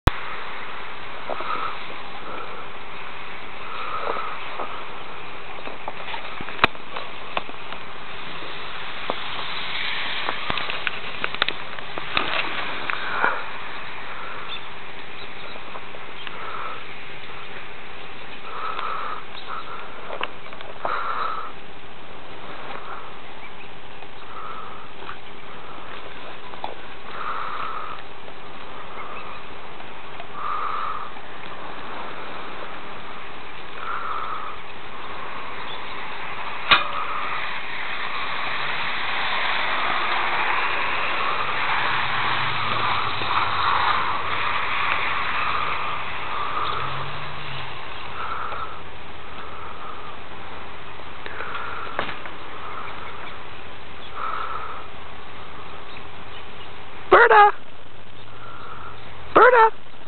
birda snow
my little dog Birda in her first snow.